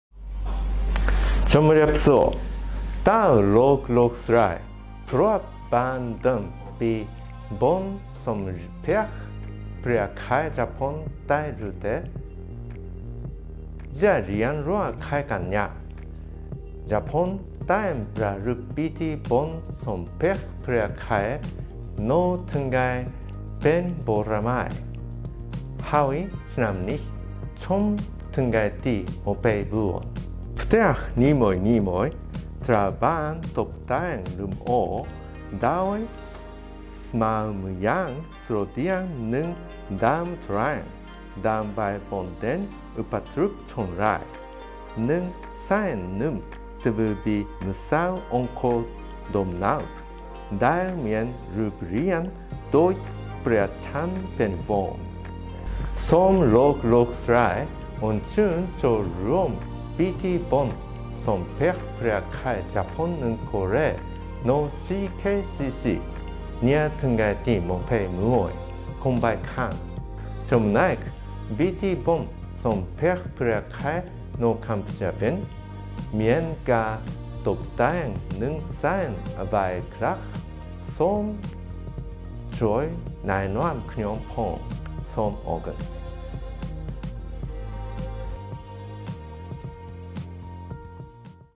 [60 seconds from Ambassador of Japan]
«៦០វិនាទីពីលោកទូតជប៉ុន»
លោកទូតនិយាយអំពីពិធីបុណ្យសំពះព្រះខែនៅប្រទេសជប៉ុន។